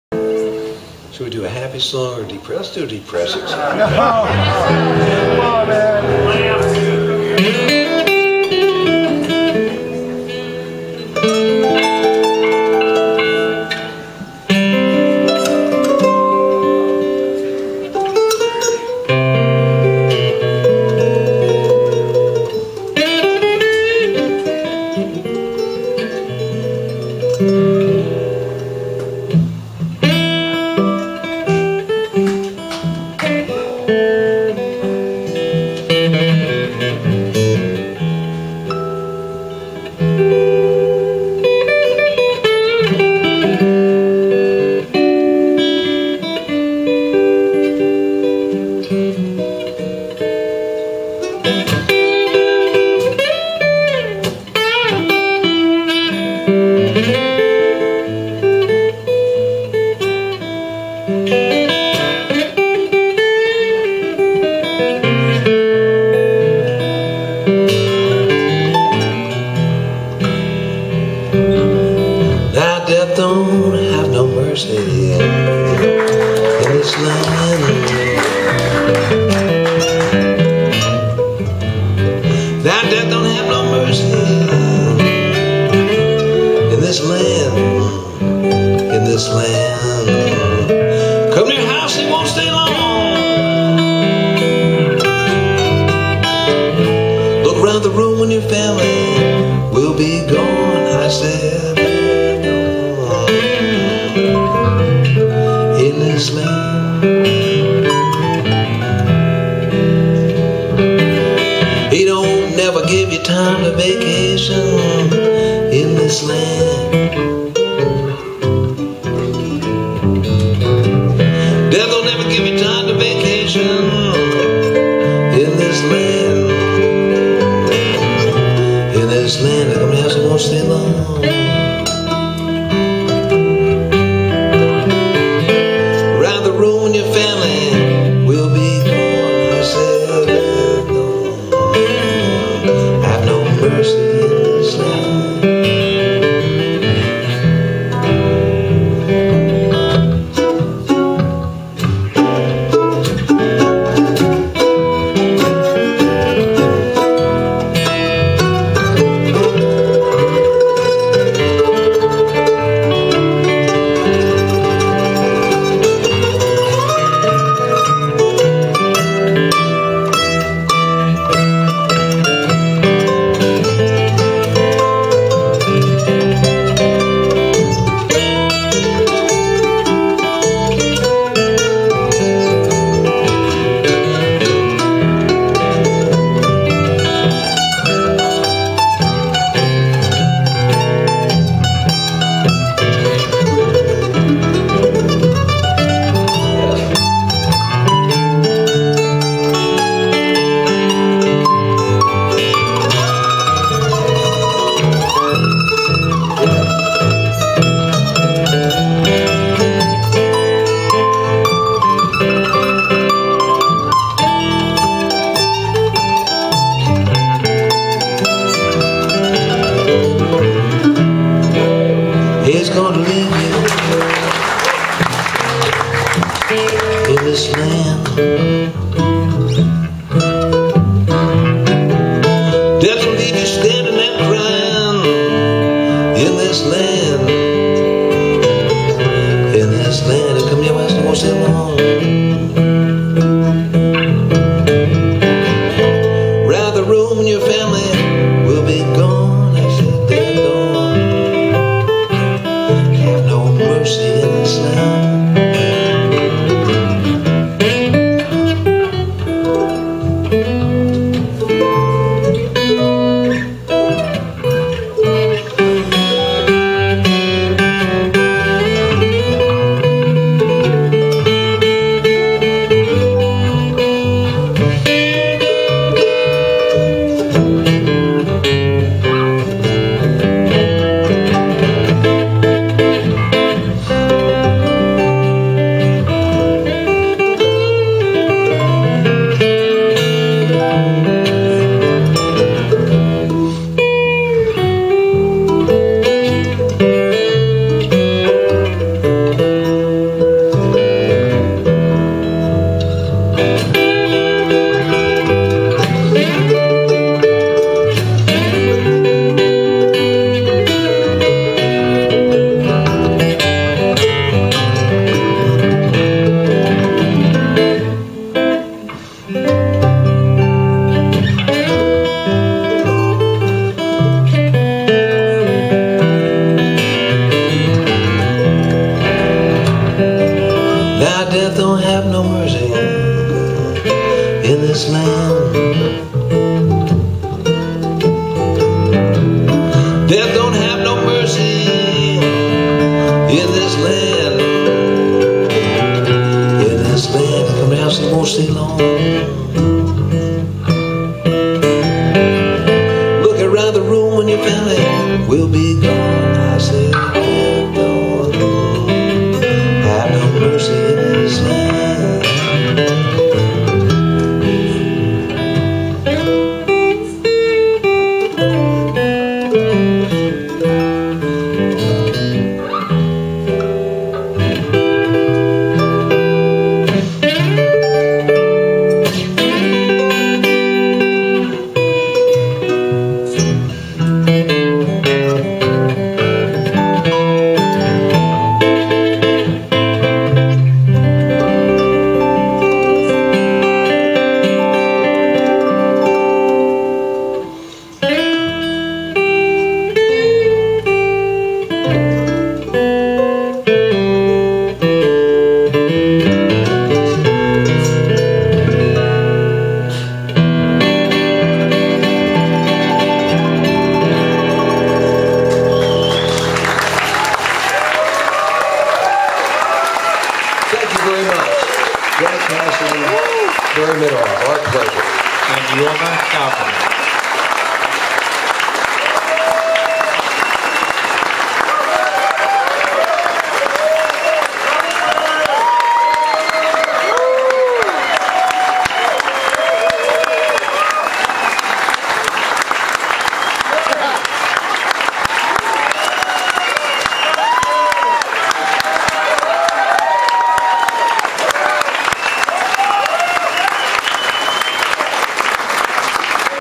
Hot Tuna ~ The Acoustic Trio at The Kessler Theater in Dallas, Texas
Jorma Kaukonen, Jack Casady & Barry Mitterhoff recorded live on July 18, 2012 at the historic Kessler Theater in X+/North Oak Cliff, Texas.